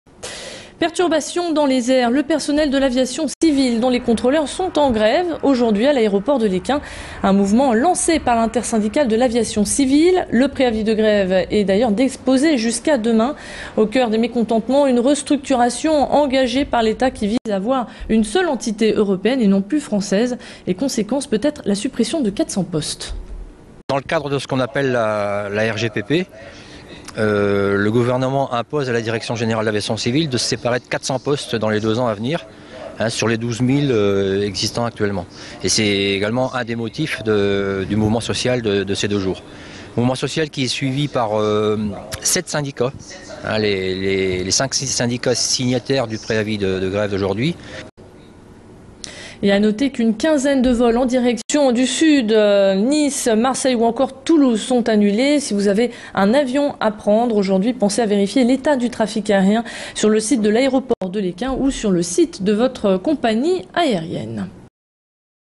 Le personnel de l'aviation civile en grève (Lille Lesquin)
Préavis de grève du personnel de l'aviation civile de l'aéroport de Lille Lesquin, dont les contrôleurs, ce qui va induire des perturbations du trafic aérien. (Droits réservés. Pour toute exploitation commerciale, veuillez nous contacter